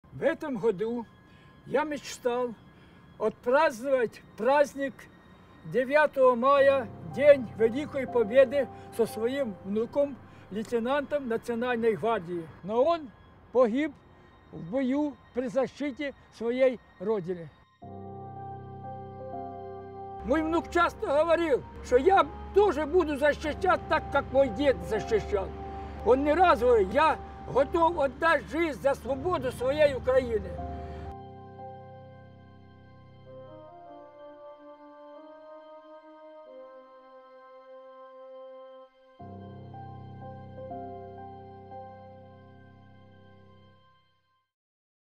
Пам’ятаємо, перемагаємо: Вічна слава героям! Патріотичне відео до Дня Перемоги
За ініціативи Уряду створені відеоісторії про роль та місце України як держави-переможниці у Другій світовій війні: «Ми хочемо, щоби вся країна побачила правду про минуле, щоби в майбутньому вона була впевнена у своїх силах», - сказав Прем’єр-міністр України Арсеній Яценюк під час презентації відеоісторій на засіданні оргкомітету з підготовки і відзначення 70-ї річниці перемоги над нацизмом у Європі.

При виробництві роликів використані матеріали:
Центрального державного кінофотофоноархіву України імені Г. С. Пшеничного
Архіву кінохроніки "British Pathé" (Великобританія)
Кримського телеканалу ATR та Ахтема Сеїтаблаєва
Телеканалу "1+1"
Телеканалу "Еспресо TV"

Приєднуйся!